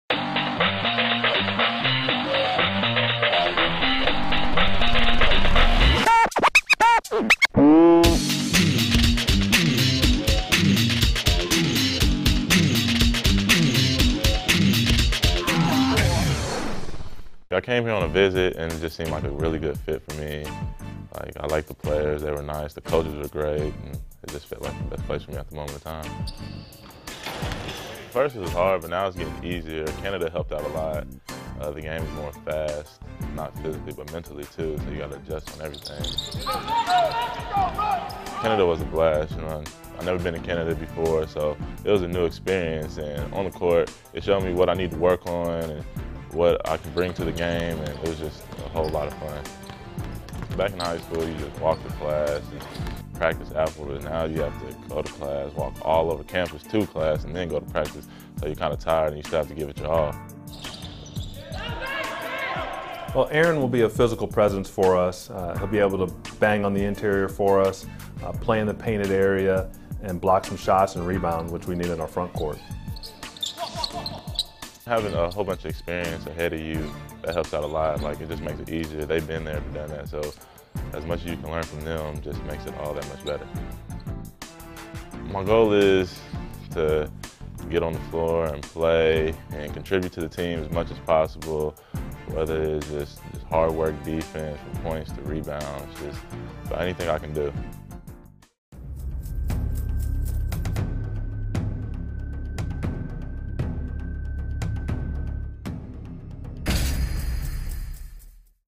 Ball State Sports Link - 12 in 12: Aaron Adeoye
Ball State Sports Link has teamed with the BSU men's basketball team for exclusive player features.

Today, meet Aaron Adeoye. Produced by Tim Keane.